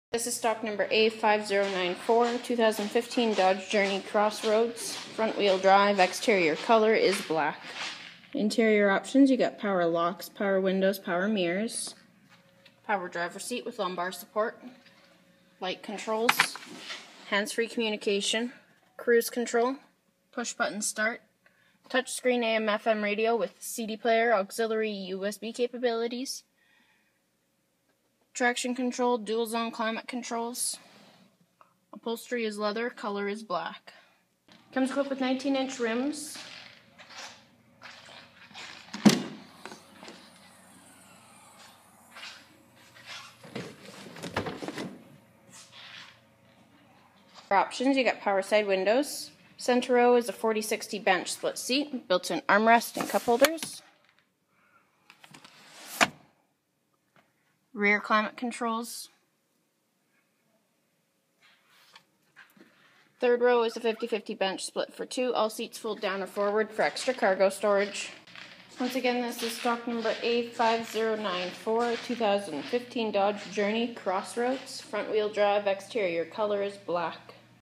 2015 Dodge Journey FWD 4dr Crossroad
This is a 2015 Dodge Journey FWD 4dr Crossroad with 6-Speed A/T transmission Black[PX8,Pitch Black Clearcoat] color and Black interior color.  This video is recorded and uploaded by cDemo Mobile Inspector